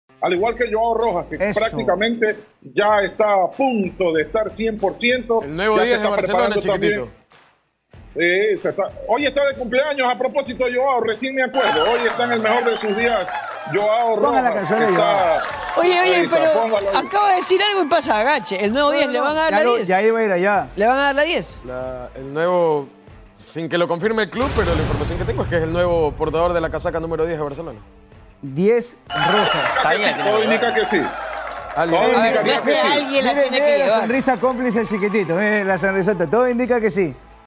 EL NUEVO 🔟 DEL ÍDOLO
🆕 EL NUEVO 🔟 DEL ÍDOLO: Según la información de Eduardo Erazo y Christian Carrasco en DeUna, Joao Rojas será el portador del número 10 en BarcelonaSC, tras la salida de Damián Díaz.

🇪🇨 Havoline, lubricante oficial de LaTri 🇪🇨